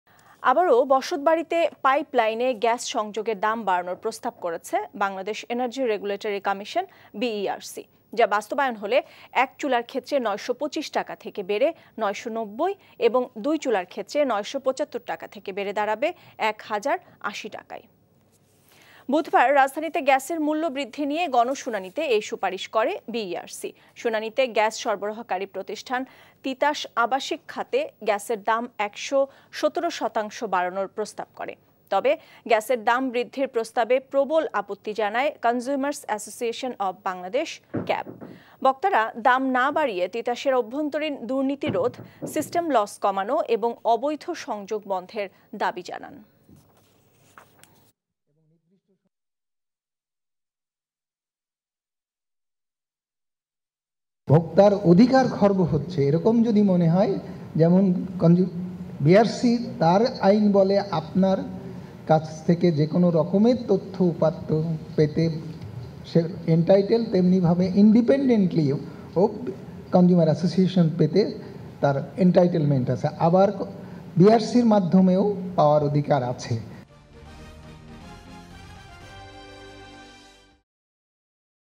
গ্যাসের দাম বাস্তবায়ন হলে এক ও দুই চুলার দাম যত টাকা হবে |Gas Prices| Petrobangla | Prices Increase
আবারও বাসাবাড়িতে পাইপলাইনে গ্যাস সংযোগের দাম বাড়ানোর প্রস্তাব করেছে বাংলাদেশ এনার্জি রেগুলারেটরি কমিশন-বিইআরসি। যা বাস্তবায়ন হলে এক চুলার ক্ষেত্রে ৯২৫ টাকা থেকে বেড়ে ৯৯০ এবং দুই চুলার ক্ষেত্রে ৯৭৫ টাকা থেকে বেড়ে দাঁড়াবে এক হাজার ৮০ টাকায়। বুধবার রাজধানীতে গ্যাসের মূল্য বৃদ্ধি নিয়ে গণশুনানিতে এ সুপারিশ করে বিইআরসি। শুনানিতে গ্যাস সরবরাহকারী প্রতিষ্ঠান তিতাস আবাসিকখাতে গ্যাসের দাম ১১৭ শতাংশ বাড়ানোর প্রস্তাব করে। তবে গ্যাসের দাম বৃদ্ধির প্রস্তাবে প্রবল আপত্তি জানায় কনজ্যুমারস অ্যাসোসিয়েশন অব বাংলাদেশ-ক্যাব। বক্তরা, দাম না বাড়িয়ে, তিতাসের অভ্যন্তরীন দুর্নীতিরোধ, সিস্টেম লস কমানো এবং অবৈধ সংযোগ বন্ধের দাবি জানান।

»» Office Address:
 42, Shaheed Sangbadik Selina Parveen Sarak, Malibag 1217 Dhaka, Dhaka Division, Bangladesh